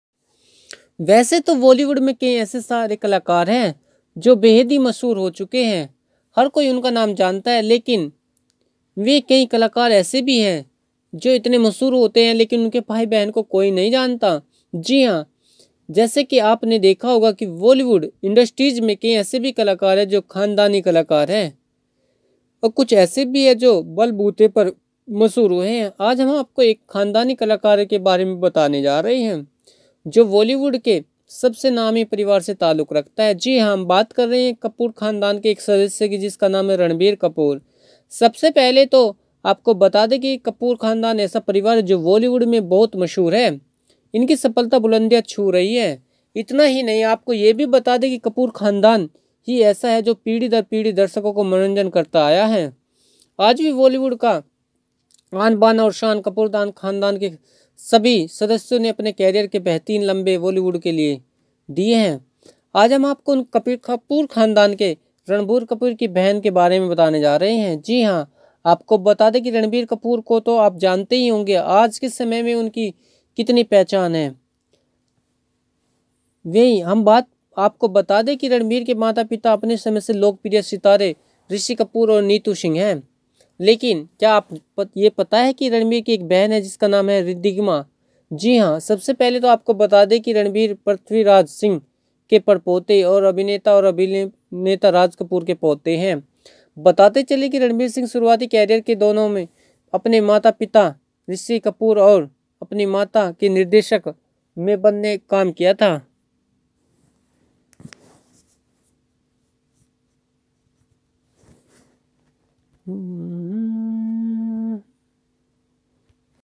ये हैं रणबीर कपूर की स्‍टाइलिश और ग्‍लैमरस बहन खुबसूरती के मामले में करीना और करिश्‍मा से भी हैं आगे
वैसे तो बॉलीवुड में कई सारे ऐसे कलाकार हैं जो बेहद ही मशहूर हो चुके हैं और हर कोई उनका नाम जानता है लेकिन वहीं कई कलाकार ऐसे भी हैं जो इतने मशहूर तो हैं लेकिन उनके भाई बहन को कोई जानता तक नहीं है। जी हां जैसा कि आप सभी ने देखा भी होगा कि बॉलीवुड इंडस्‍ट्री में कई ऐसे भी कलाकार है जो खानदानी कलाकार में गिने जाते हैं और कुछ ऐसे भी हैं जो अपने बलबूते पर मशहूर हुए हैं। आज हम आपको एक खानदानी कलाकार के बारे में बताने जा रहे है जो बॉलीवुड के सबसे नामी परिवार से ताल्‍लुक रखता है जी हां हम बात करने जा रहे हैं कपूर खानदान के एक सदस्‍य की जिनका नाम है रणबीर कपूर।

सबसे पहले तो आपको ये बता दें कि कपूर खानदान ऐसा परिवार है जो पूरे बॉलीवुड में बहुत मशहूर है और इनकी सफलता बुलंदियों को छू रही है इतना ही नहीं आपको ये भी बता दें कि कपूर खानदान ही ऐसा है जो पीढ़ी दर पीढ़ी दर्शको का मनोरंजन करता आया है और आज भी बॉलीवुड की आन बान और शान है। कपूर खानदान के सभी सदस्यों ने अपने करियर के बेहतरीन लम्हे बॉलीवुड को दिए।